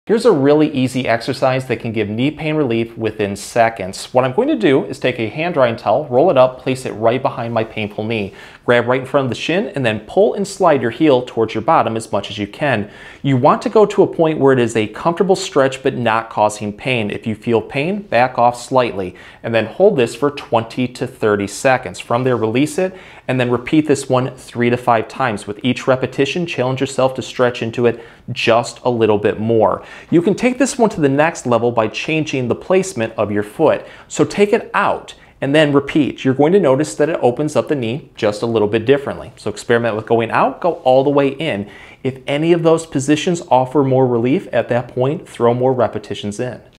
How to Relieve Knee Pain in Seconds
Dr. Rowe shows an easy exercise that can give knee pain relief within seconds.

This exercise will help traction open the knee, relieving pressure and tension. It can be done throughout the day at home, and only requires only a small towel.